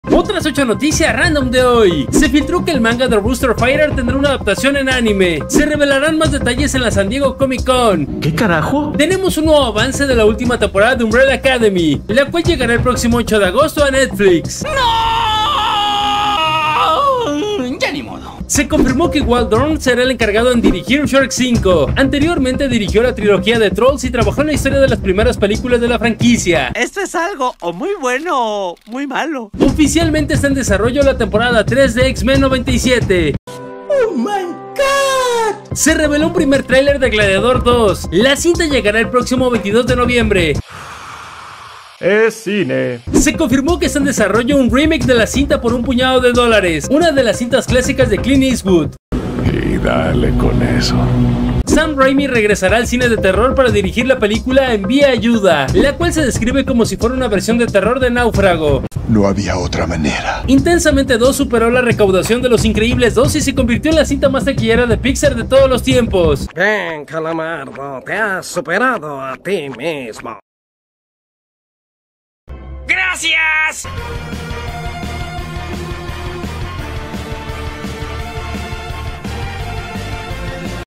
SHREK 5, anime ROOSTER FIGHTER, GLADIADOR 2, SAM RAIMI regresa al terror, X-MEN 97 T3 y MÁS
Otras 8 noticias random

noticias ultimahora videojuegos películas cine otrasnoticiasrandom noticiasrandom